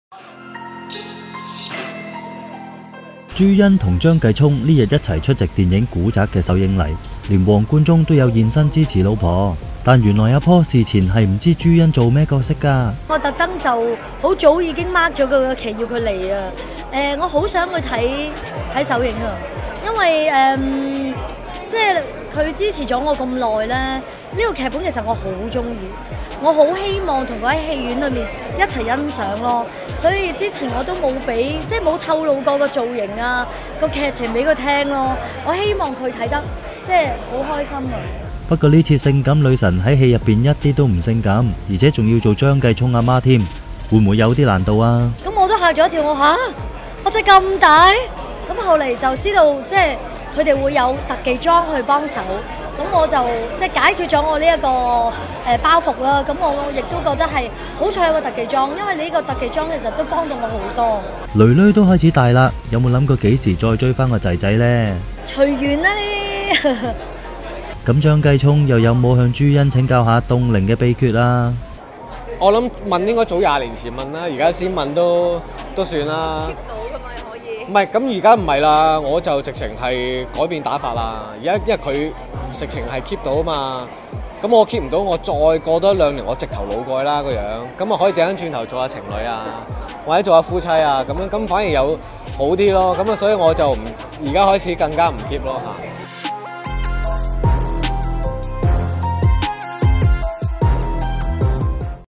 【娛樂】新片做張繼聰媽媽 朱茵 : 我都嚇一跳 2018-09-28
朱茵與張繼聰出席電影《古宅》首映禮，朱茵老公Paul也有撐場。

朱茵表示 :「初時接到劇本，真係嚇一跳，我邊有咁大個仔。好彩有特技化妝，真係幫到我好多。」

universalproductionmusic